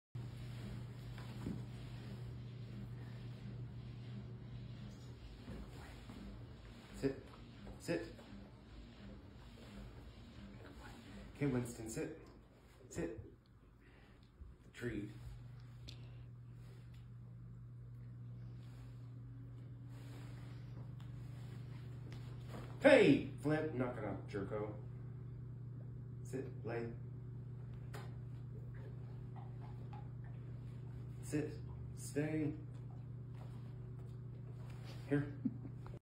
When you're trying to train the puppy but everyone wants in on the action
The bunny is the star of the show!

Submitted by Camille Hurst in @DoggoHumor  on Facebook